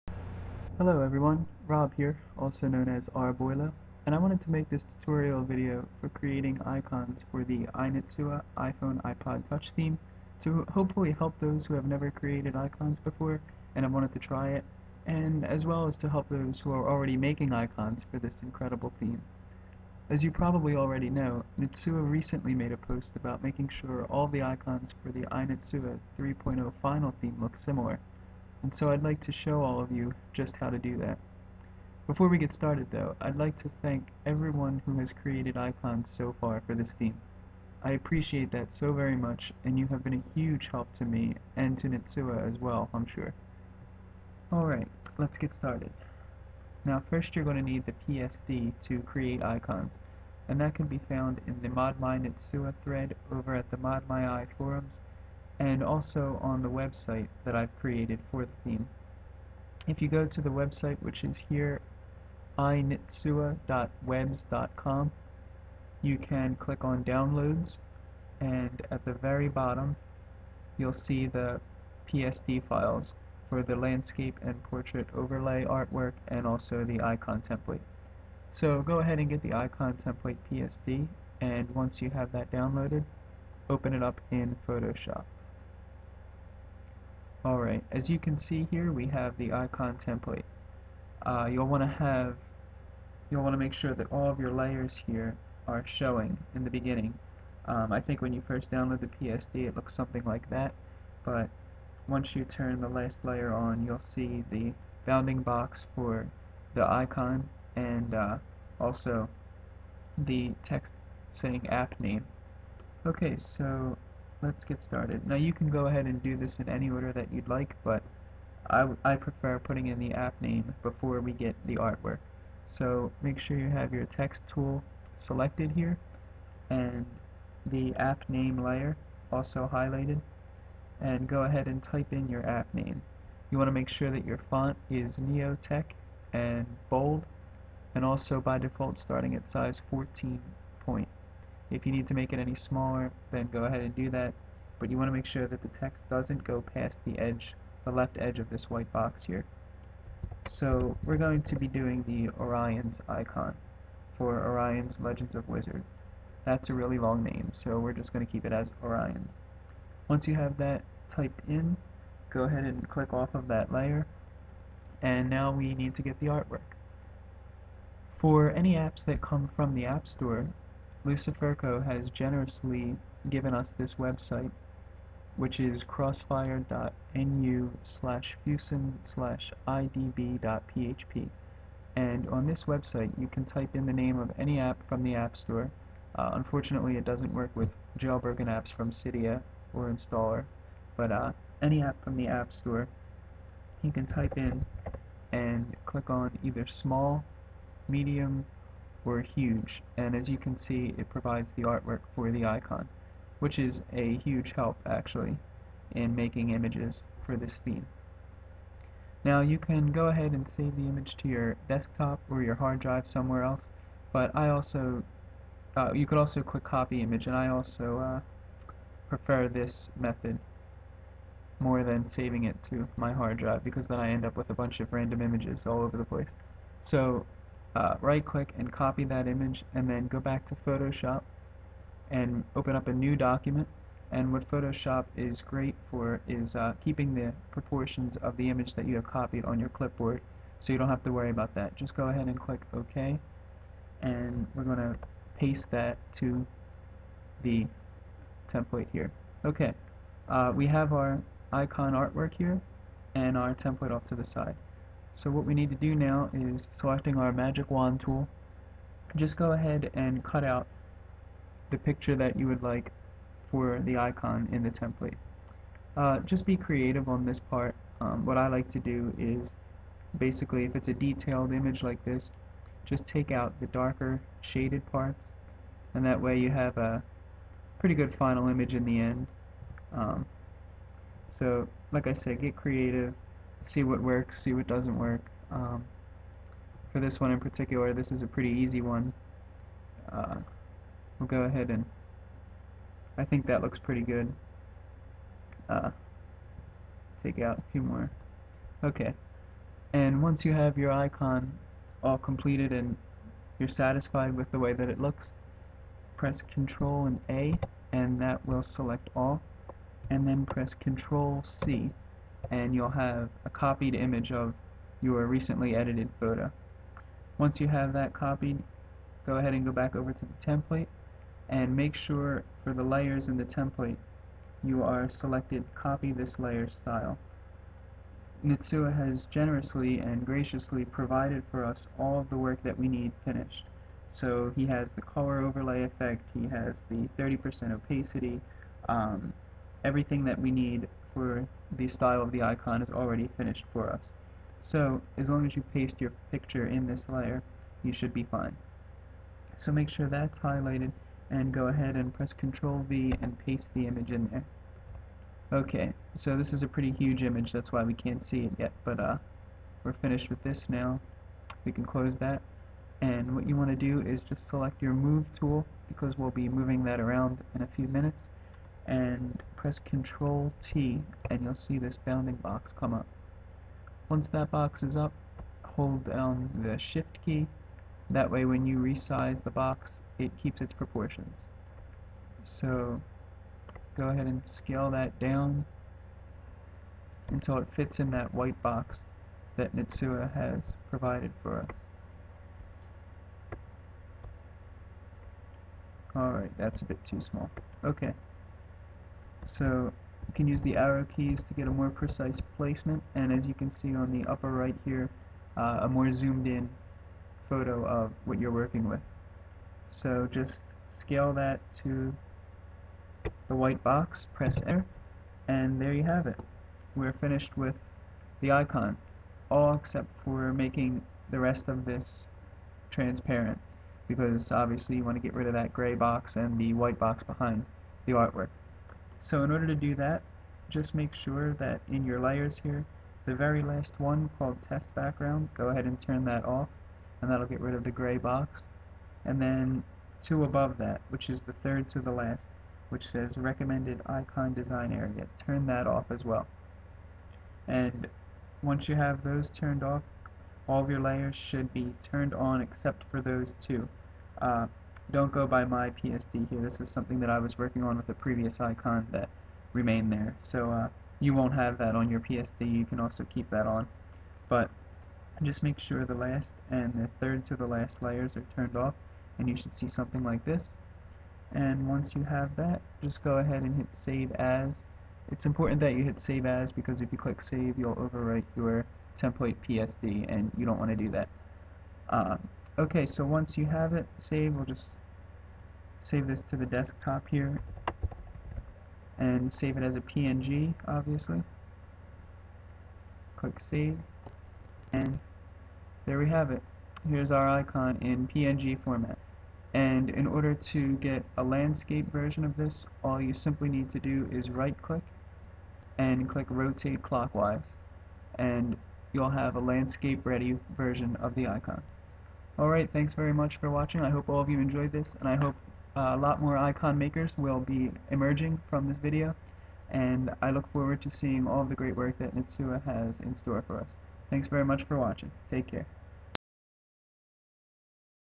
iNitsua Icon Tutorial
A short tutorial video explaining how to create icons for the iNitsua iPhone/iPod Touch theme. The theme can be downloaded here:

If you enjoy this theme, please rate the thread 5 stars over the the ModMyi forums, and consider donating to k.nitsua for his incredible work!